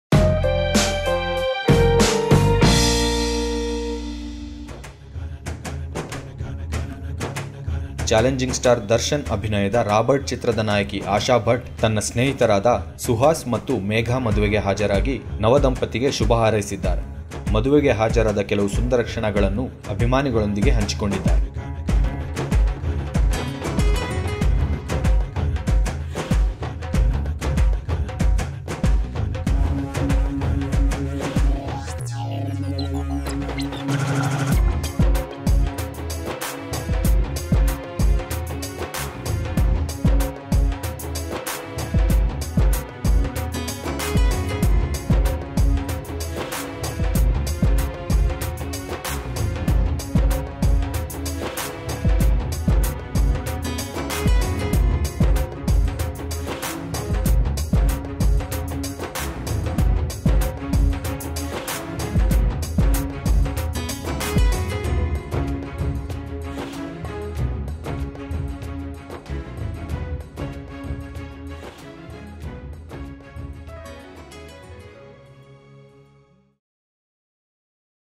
ರಾಬರ್ಟ್ ನಟಿ ತನ್ನ ಸ್ನೇಹಿತರ ಮದುವೆಯಲ್ಲಿ | Roberrt fame Asha Bhat in friends marriage | kannadaTv
challenging star darshan's roberrt movie heroine Asha Bhat attends feiends wedding.